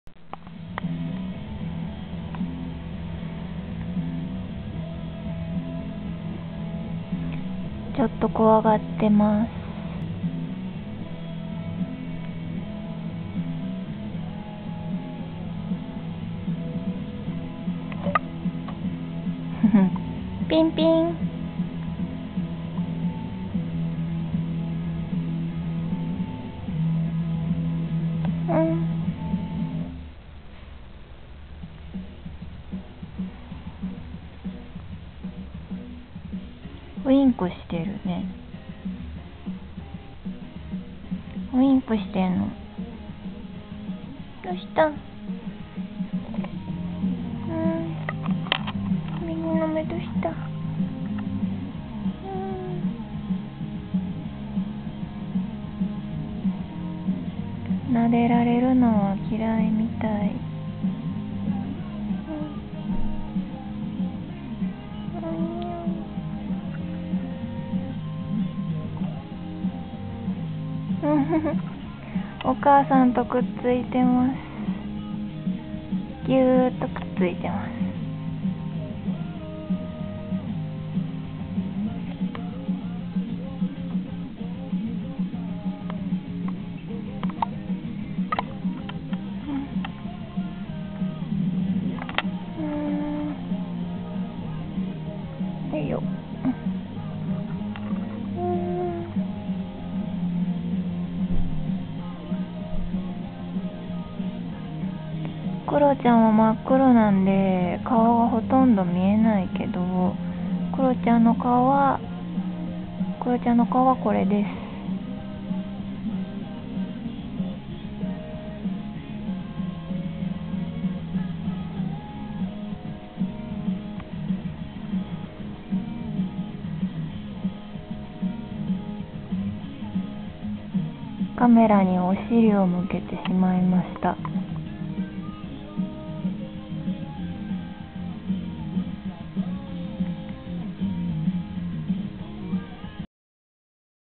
クロちゃんと子猫トラ２.MOV
マンションに住み着いていたクロちゃんとその子猫黒白。

それぞれ良い里親さんにもらわれていきました。